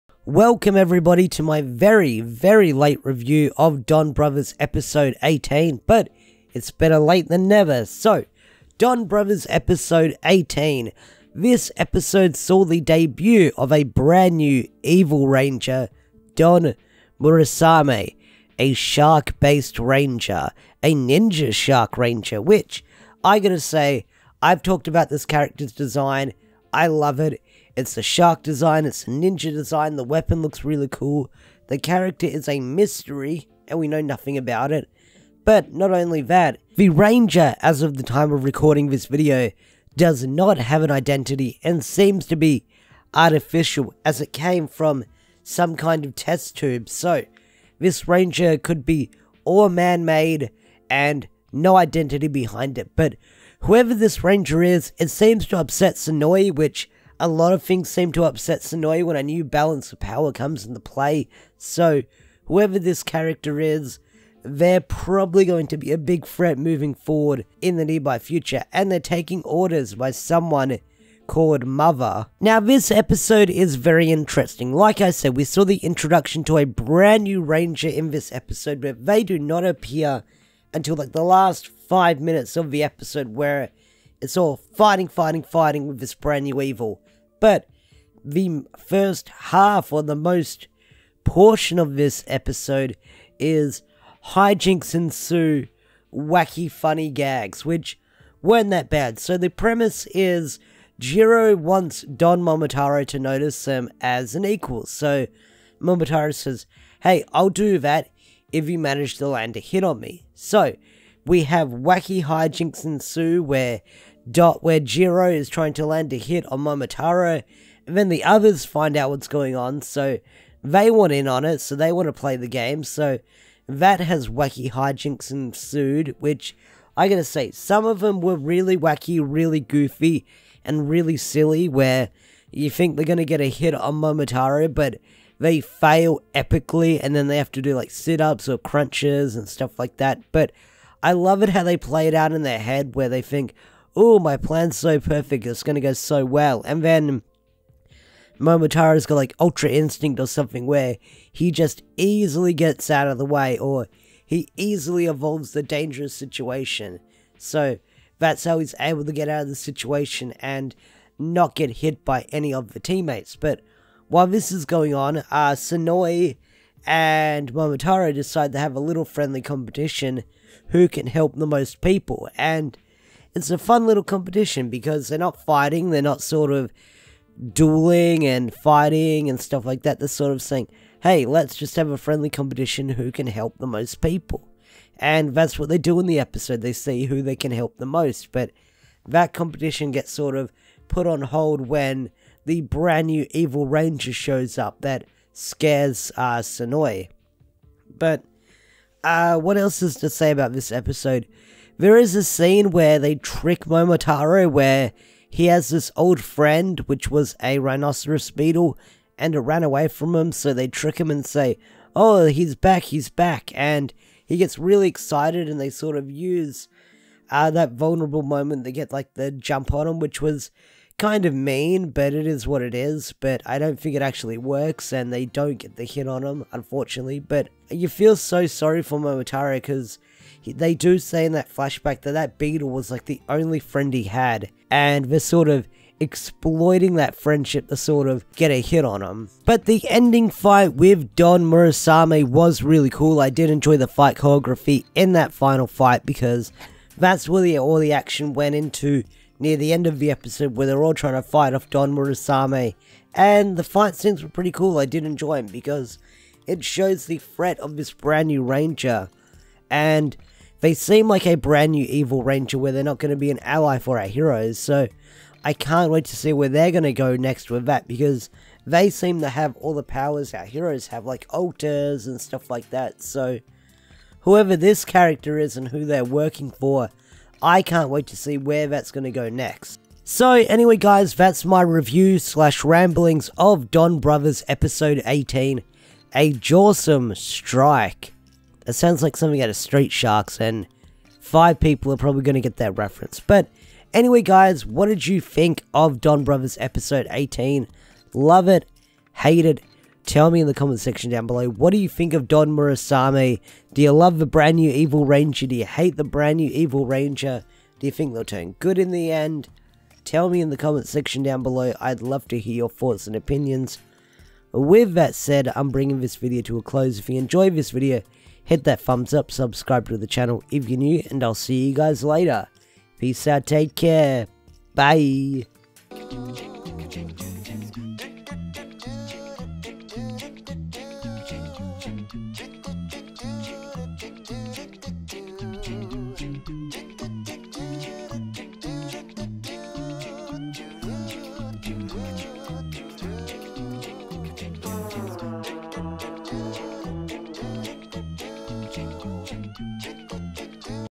Avataro Sentai Donbrothers Episode 18 REVIEW: A Jawsome Strike
The other Donbrothers are not happy that Jiro is now officially part of the team. Jiro, who is not satisfied that he is just a mere companion, tells Taro about it and challenges him that if Jiro is able to hit him once, Taro will acknowledge him as his equal. The others heard about this and decided to participate. Meanwhile, Don Murasame is awakened by the voice of a mysterious Mother and appears in front of the Donbrothers.